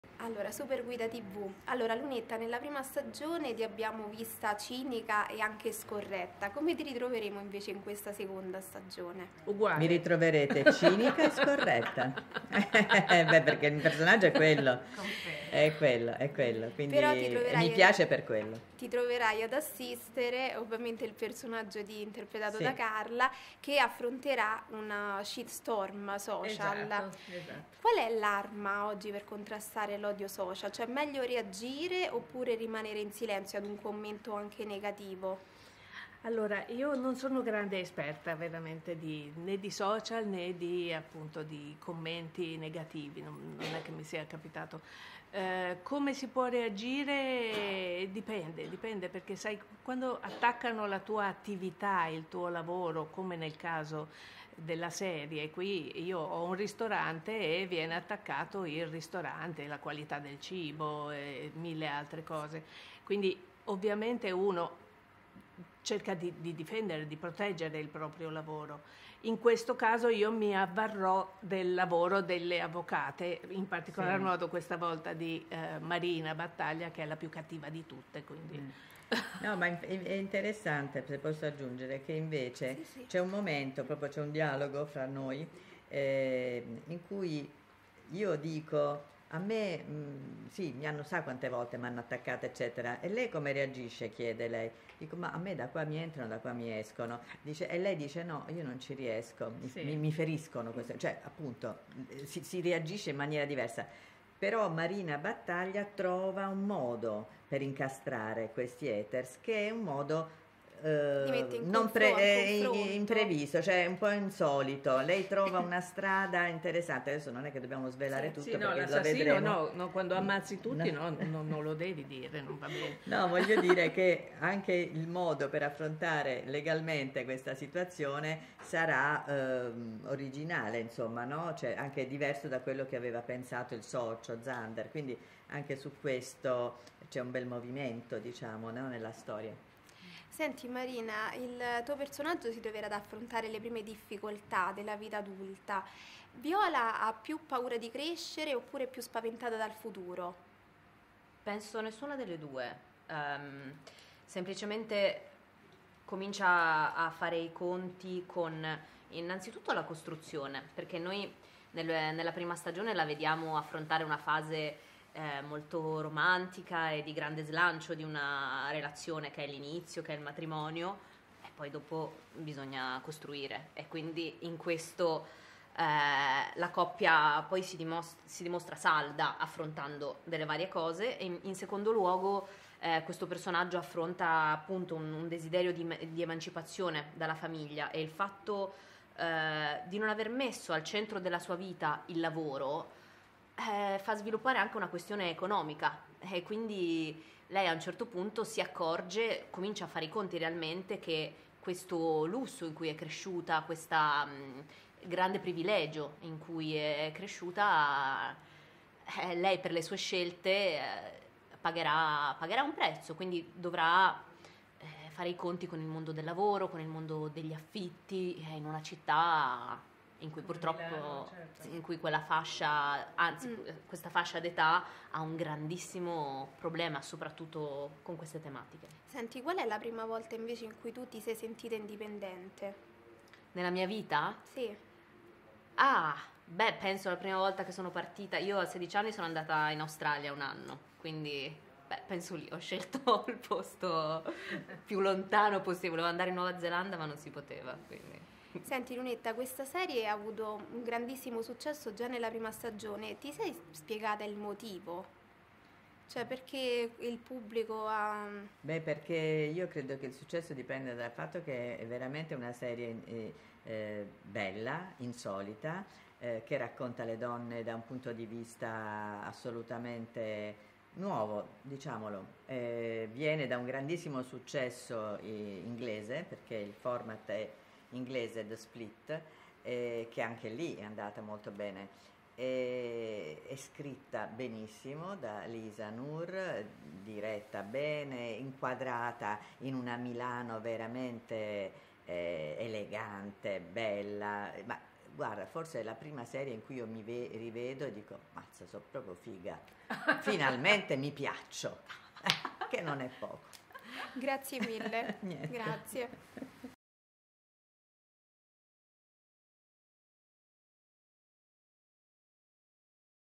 "Studio Battaglia 2", intervista esclusiva a Lunetta Savino, Carla Signoris e Marina Occhionero
Studio Battaglia torna dal 19 marzo in prima serata su Raiuno con tre imperdibili puntate. In questa seconda stagione, Marina si ritroverà ad affrontare assieme alle figlie scelte difficili e nuovi drammi. Le Battaglia dovranno soprattutto ambientarsi al grande cambiamento visto che lo studio legale si è appena fuso con quello di Zander. La nostra intervista esclusiva a Lunetta Savino, Carla Signoris e Marina Occhionero.